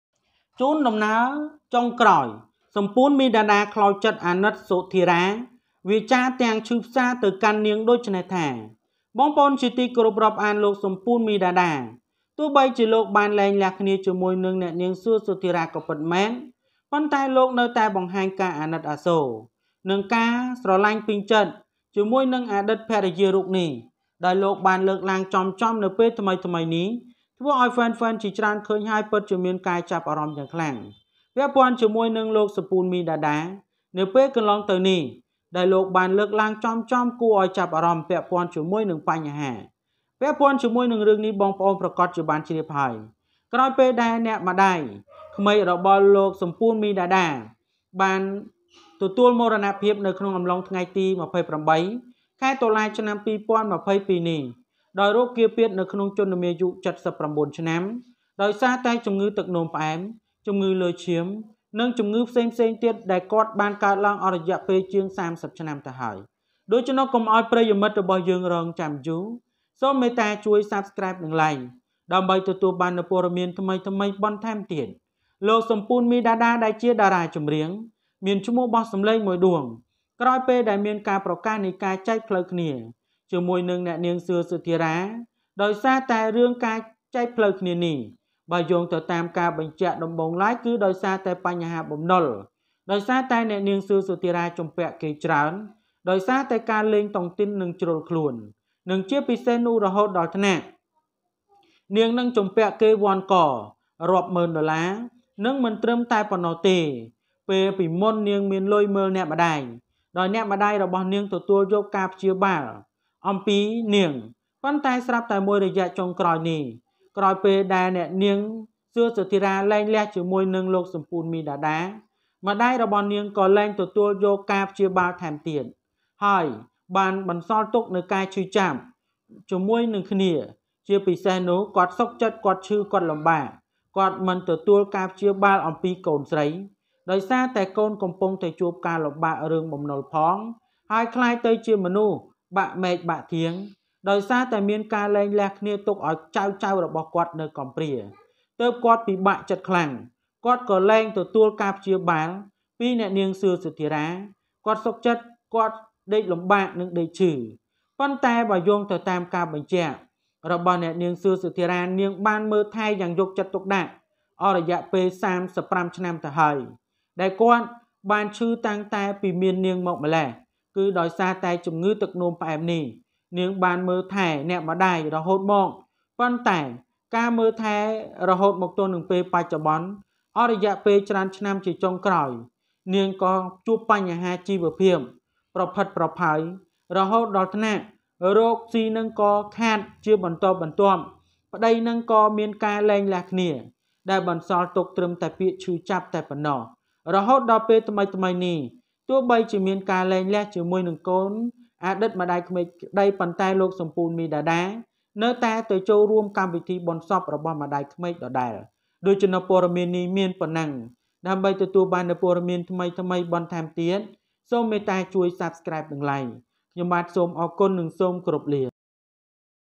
ជួនដំណើរចុងក្រោយ សាពូន មីដាដា ខ្លោចចិត្ត អាណិត សុធារ៉ា វាចារទាំងឈឺផ្សារ ទៅកាន់នាងថា
ជួនដំណើរចុងក្រោយ សាពូន មីដាដា ខ្លោចចិត្ត អាណិត សុធារ៉ា វាចារទាំងឈឺផ្សារ ទៅកាន់នាងថា On the last trip, Sapoun Midada felt sorry for Sotheara, and she said to her in pain.